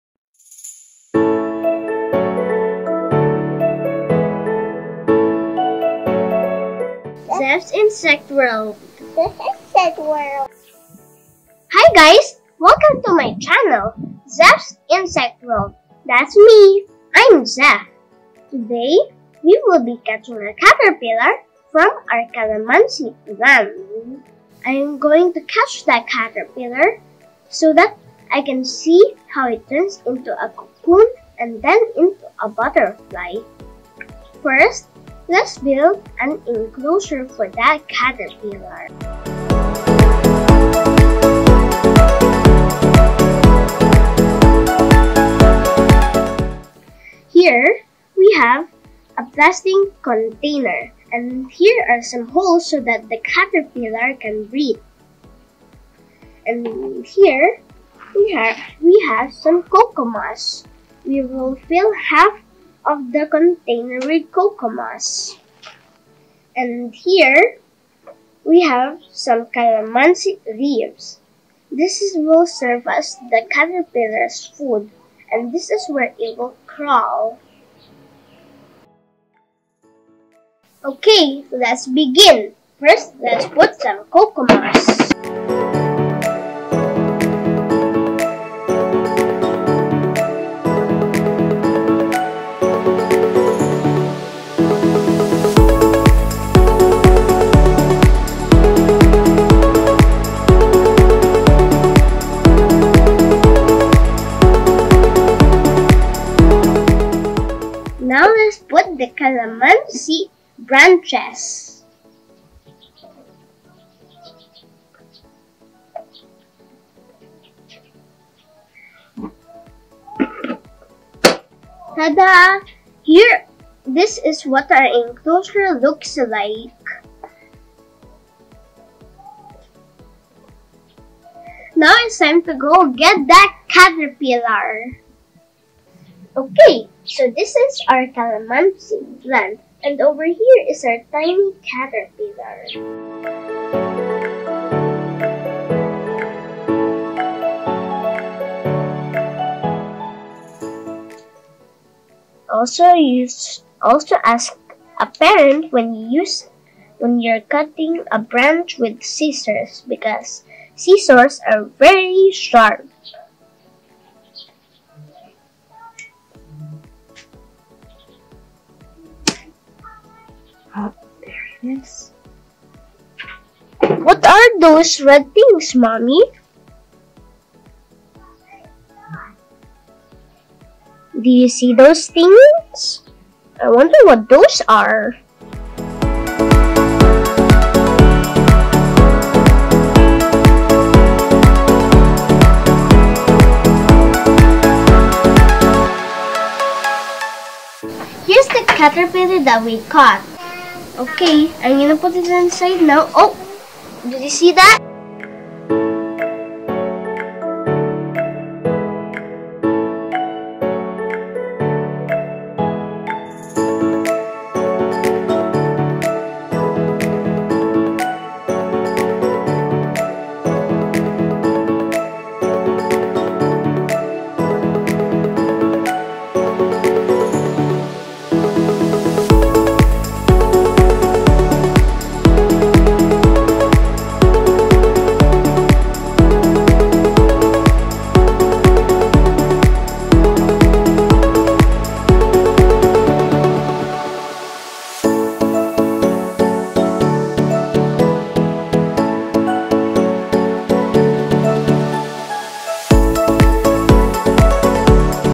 My First Caterpillar
Watch me catch the caterpillar from our calamansi plant! I'll be taking care of this caterpillar until it becomes a butterfly.